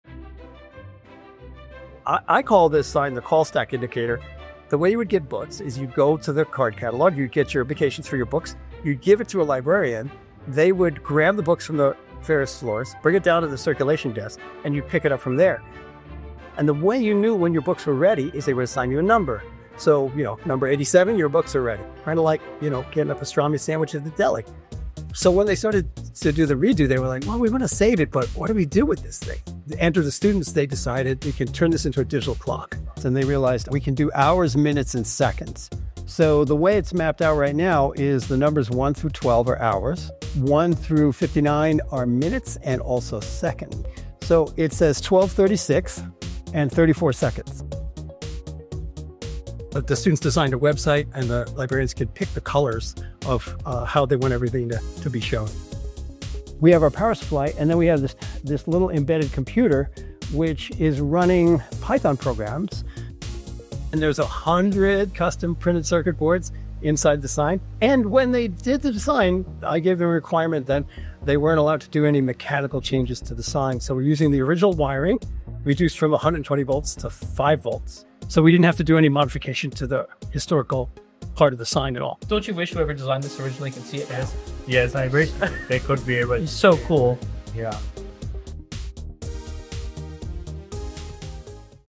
Cornell Engineers Turn Vintage Library Tech into Digital Clock
Cornell Engineering students are transforming a historic call board in Olin Library into a fully functional digital clock. Using Python, custom circuit boards, and the original 1960s wiring, and guided by Professor of Practice Joe Skovira the team is preserving the past while engineering the future.